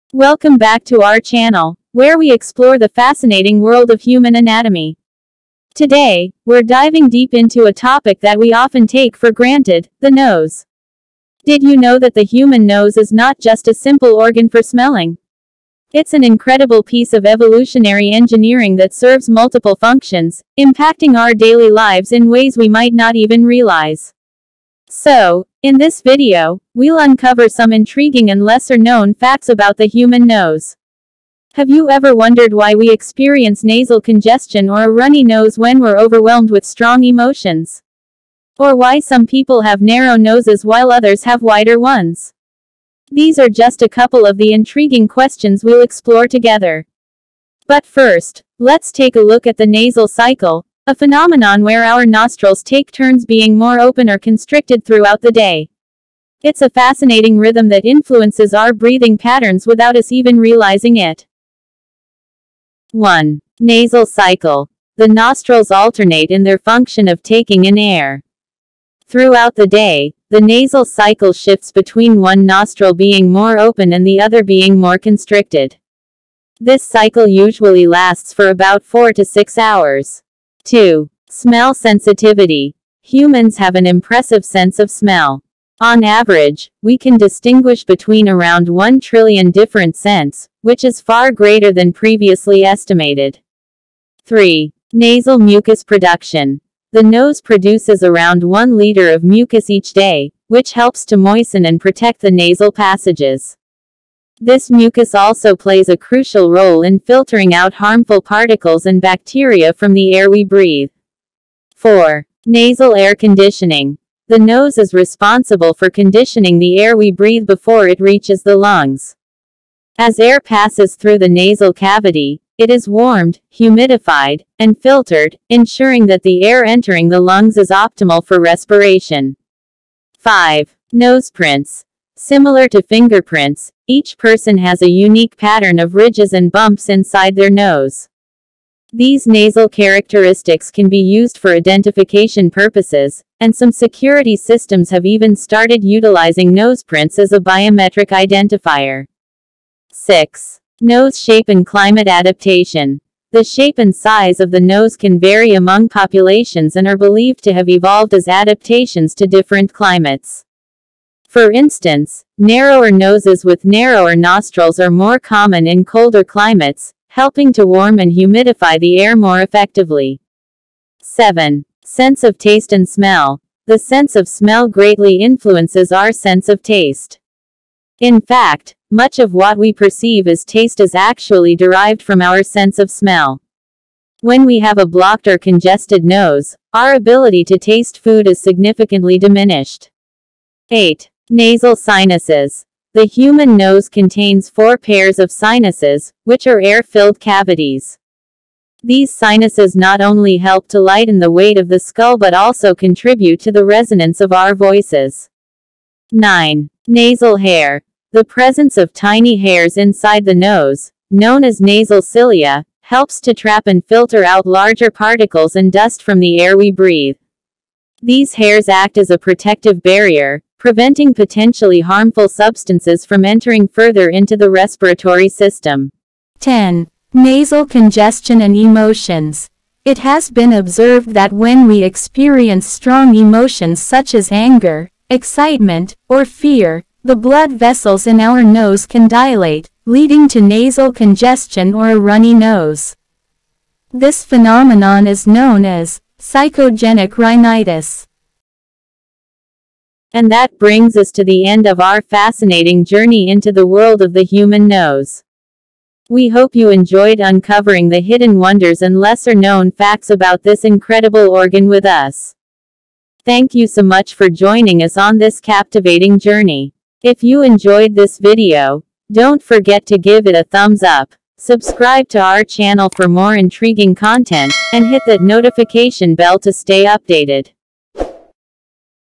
Facts of Human Nose
The human nose is a fascinating organ that serves multiple functions beyond just the sense of smell. It undergoes a nasal cycle where one nostril is more open than the other throughout the day. Humans possess an exceptional sense of smell, capable of distinguishing a vast number of scents. The nose produces mucus to moisturize and safeguard the nasal passages, while also filtering out harmful particles. It plays a crucial role in conditioning the air we breathe, warming, humidifying, and filtering it before it reaches the lungs. The shape of the nose can vary among populations, potentially adapting to different climates. Nasal characteristics, similar to fingerprints, can be used for identification. The nose contains sinuses, aiding in voice resonance and reducing skull weight. Nasal hair helps trap particles, and dilated blood vessels in the nose can cause congestion during intense emotions. Understanding these lesser-known facts highlights the complexity and importance of the human nose.